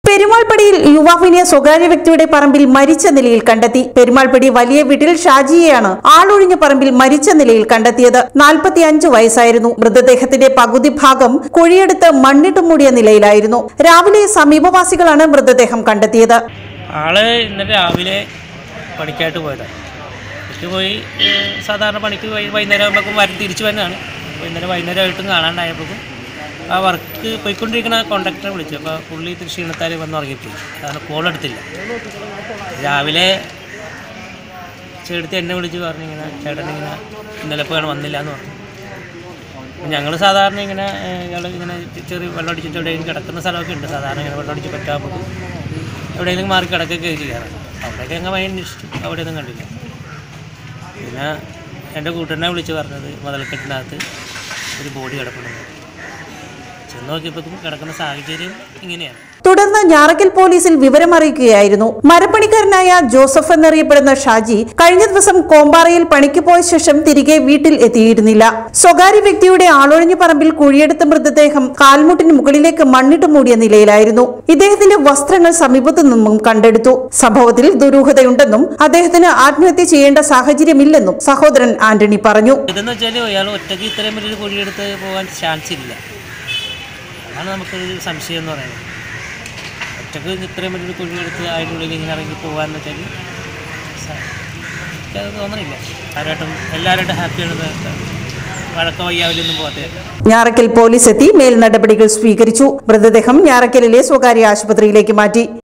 പെരുമാൾപ്പടിയിൽ യുവാവിനെ സ്വകാര്യ വ്യക്തിയുടെ പറമ്പിൽ മരിച്ചനിലയിൽ കണ്ടെത്തി.
പെരുമാൾപ്പടി വലിയവീട്ടിൽ ഷാജിയെയാണ്  ആളൊഴിഞ്ഞ പറമ്പിൽ മരിച്ച നിലയിൽ കണ്ടെത്തിയത്